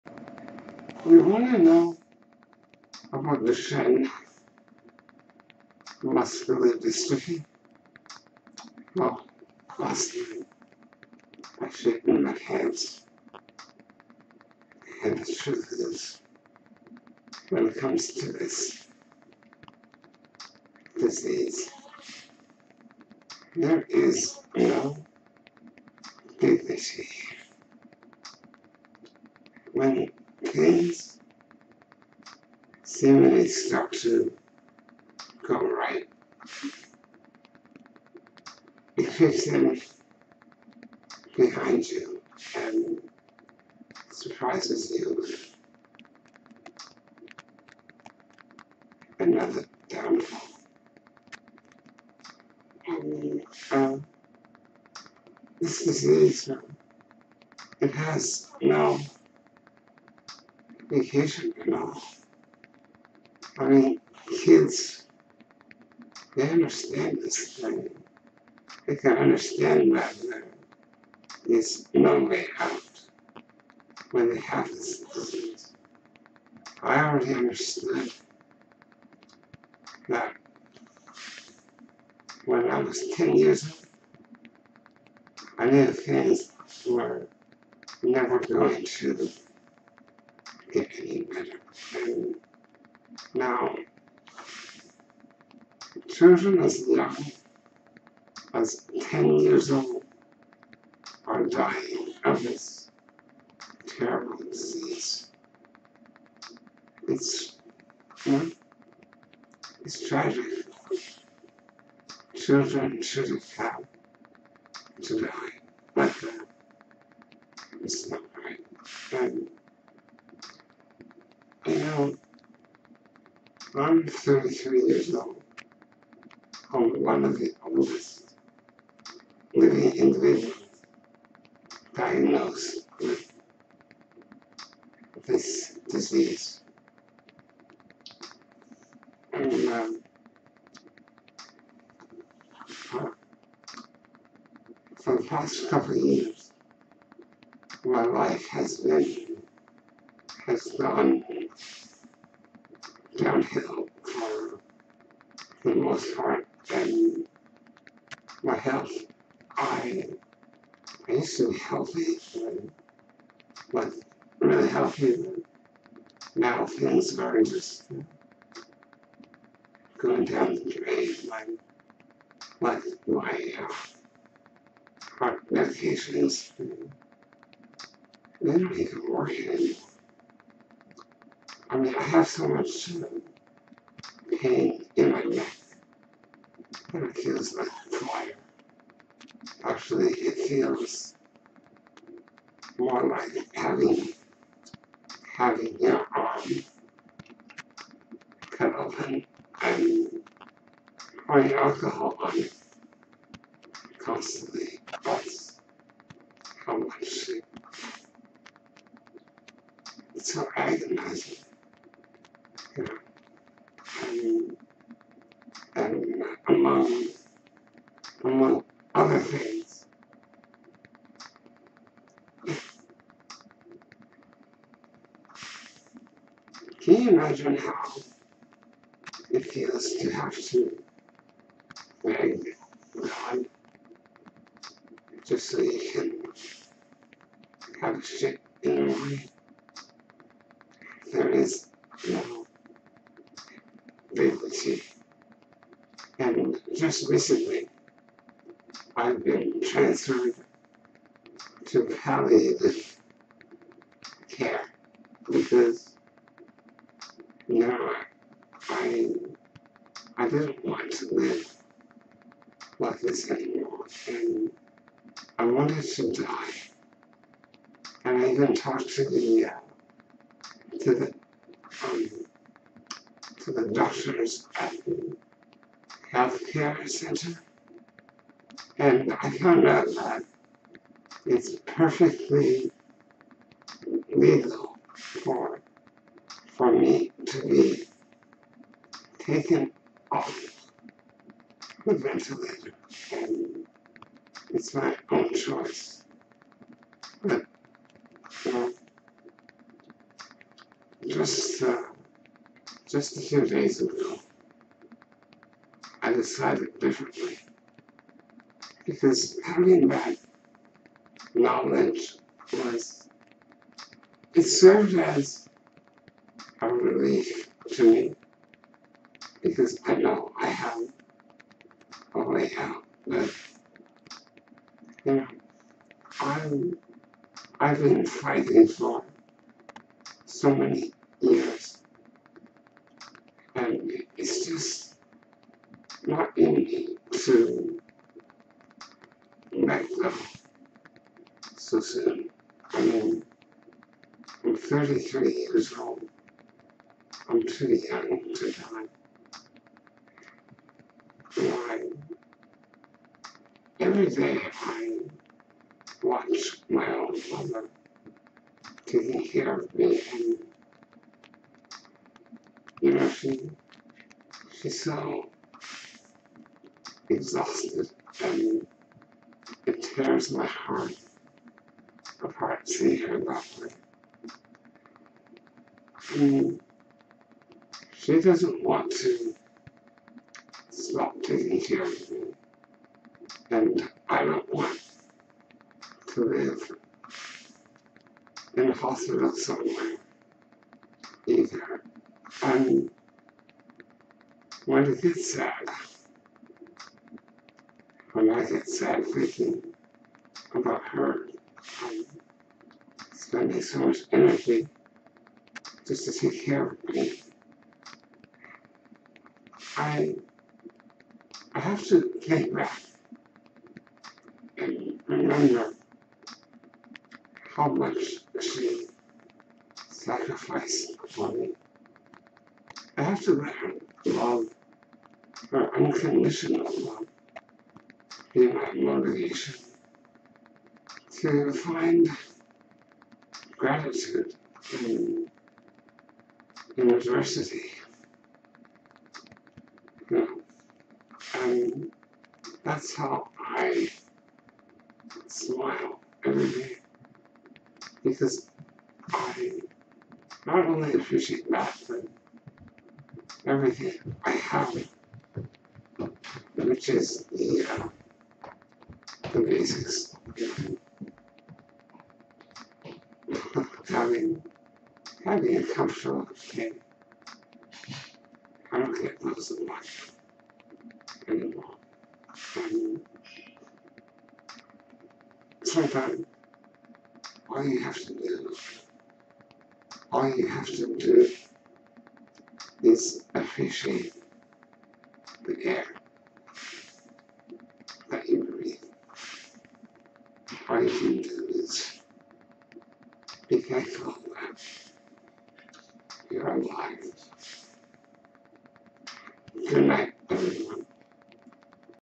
You Want to Know about Duchenne Muscular Dystrophy?
Duchenne Muscular Dystrophy is the #1 killer of children in the world.  Approximately 1 out 3,500 boys are diagnosed with this fatal illness, worldwide.  33-year-old, Ricky Tsang, speaks.